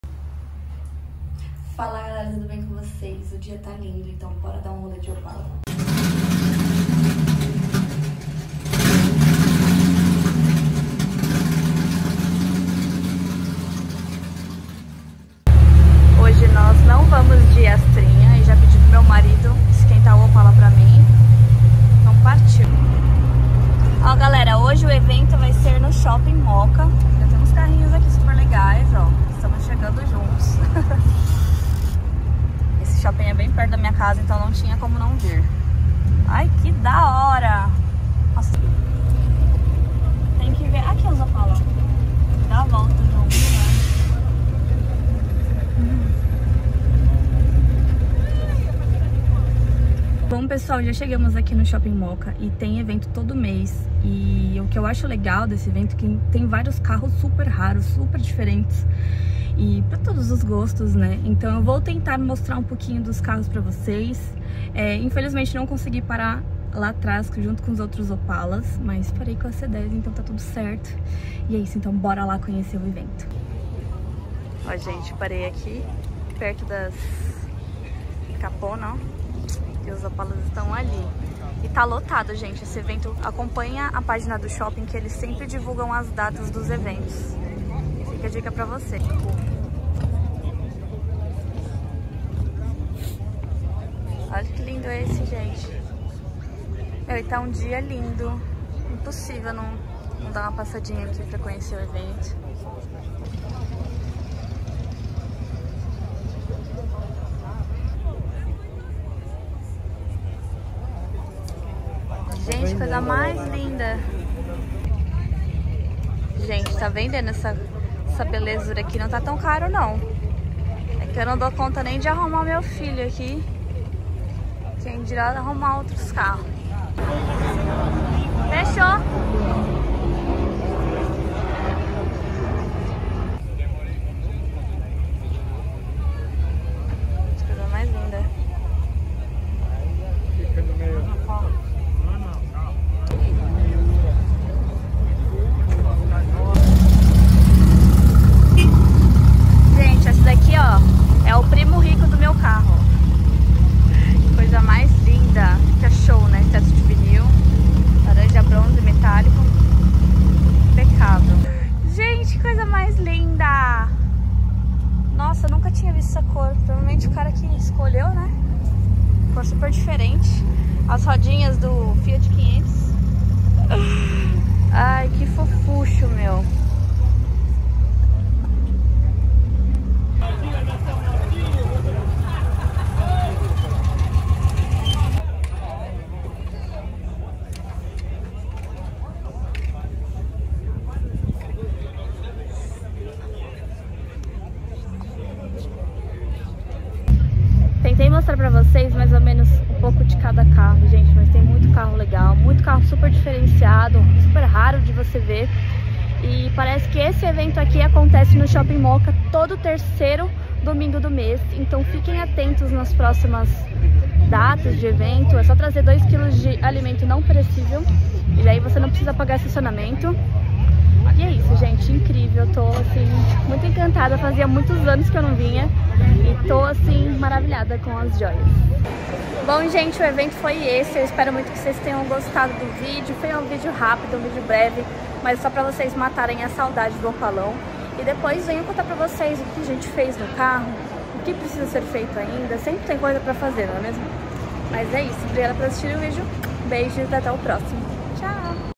Rolê com ele ❤️
Fala lindezas , tudo bem ??
O opala está de volta e fomos num evento bem legal e já fica de dica para vocês !!

Todo 3º domingo do mês lá no shopping da Mooca / SP